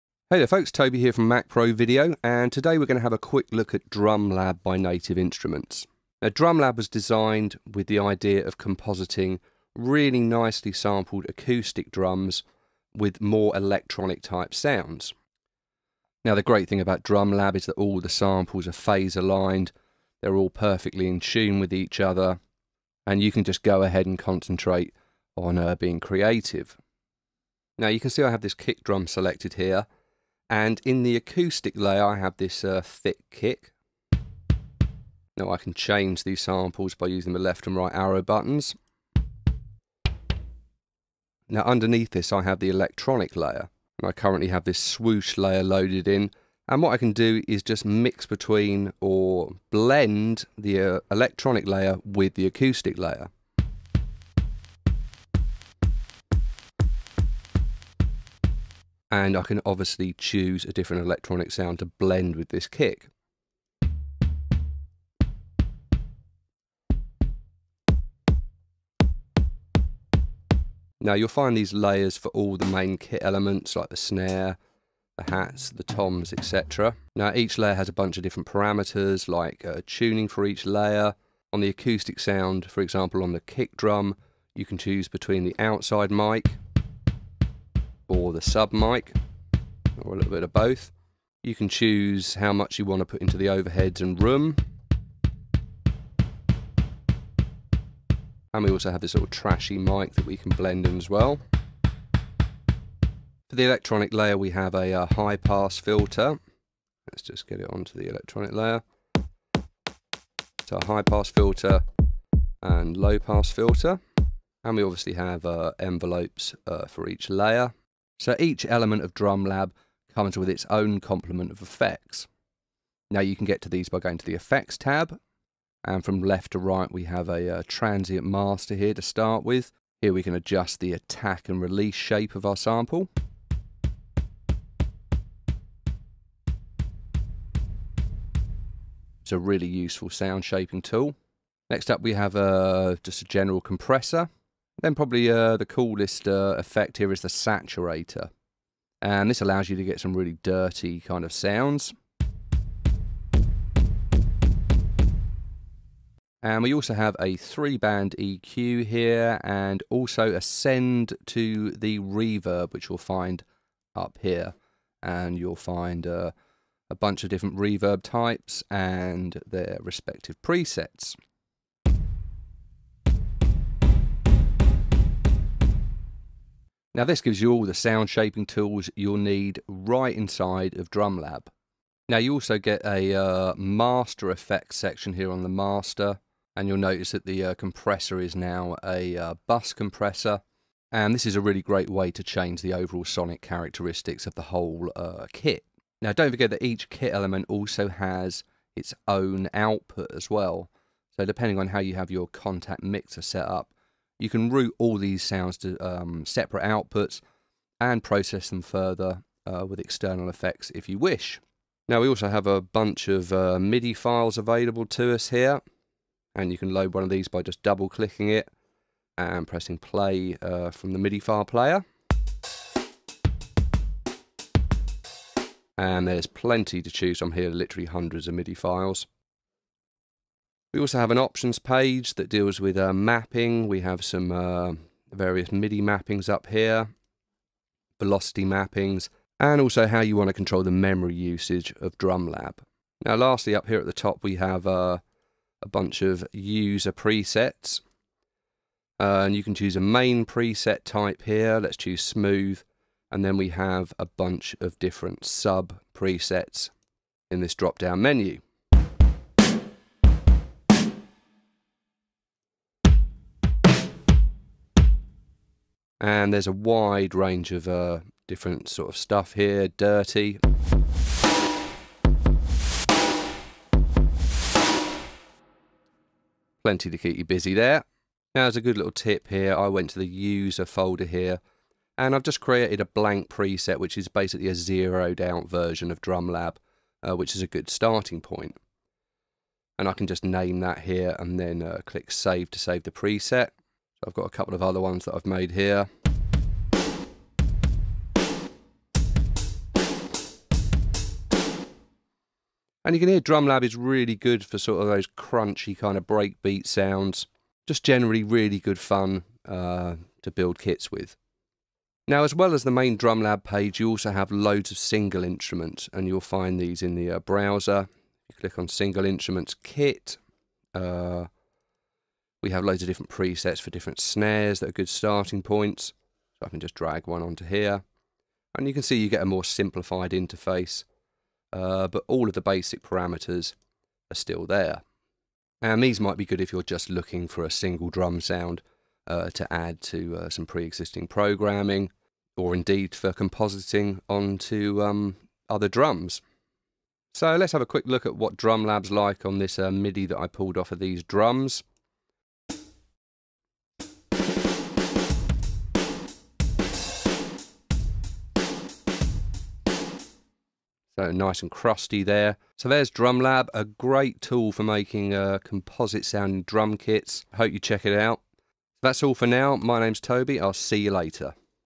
DrumLab: An Introduction
Toby Pitman explores the interface and features in Native Instruments' new DrumLab.